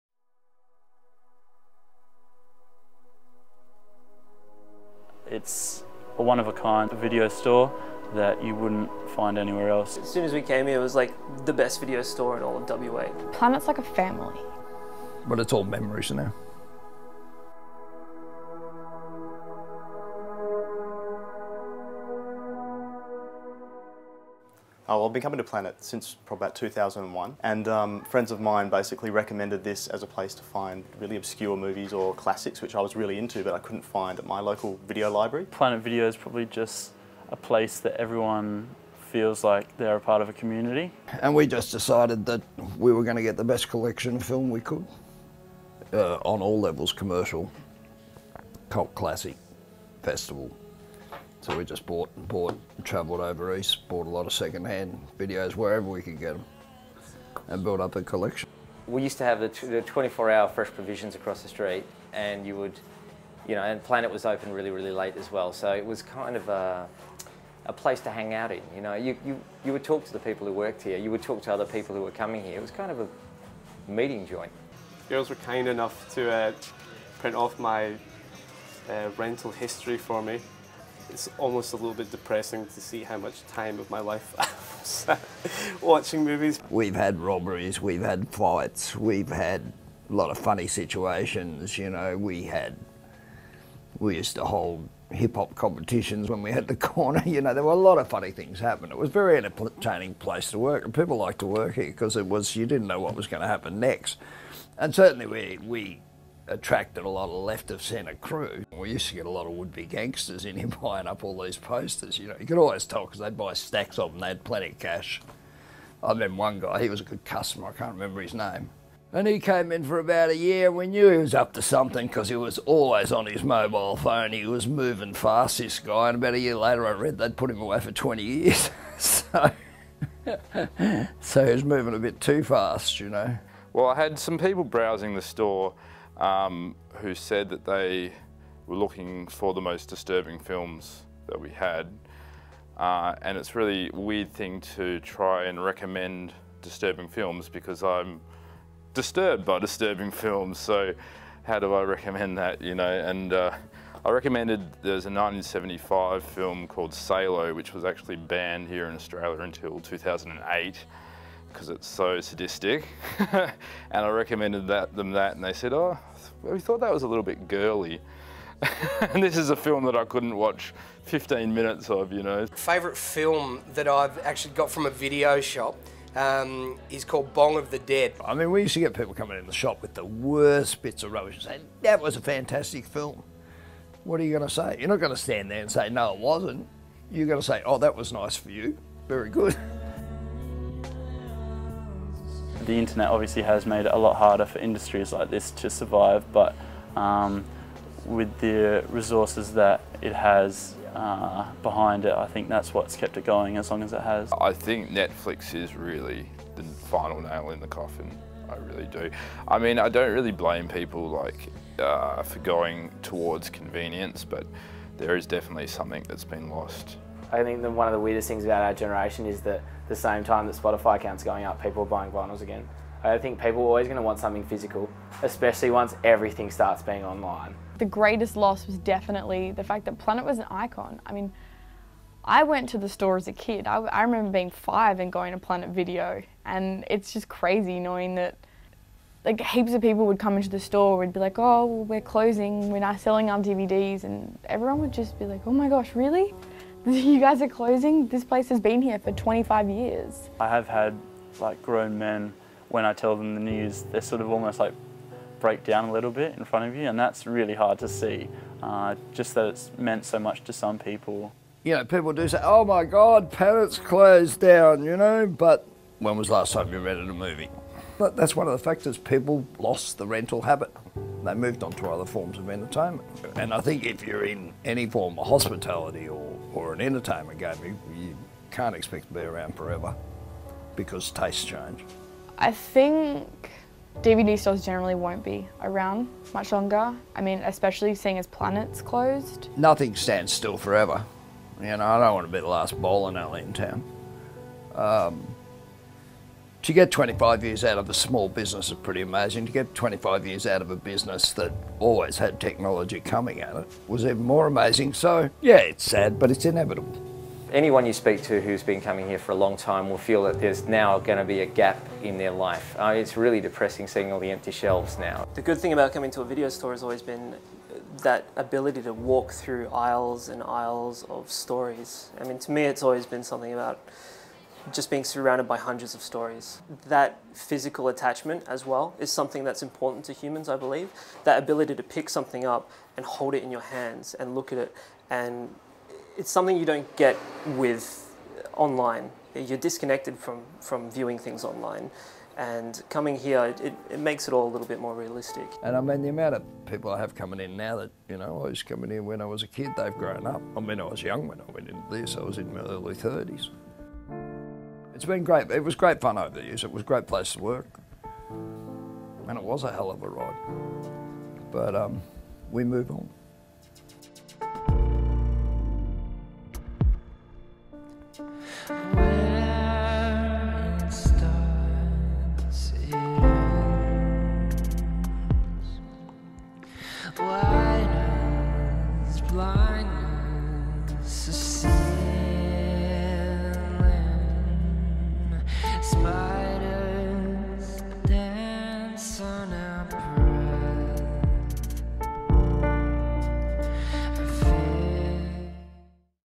Empty Shelves
25 years of crazy customers and twisted movies made Planet Video an iconic part of Perth’s pop culture scene and was often referred to as having the best film range in the country. But with Netflix and Internet Piracy now the norm, the video store's ride was over. We were there for its final days.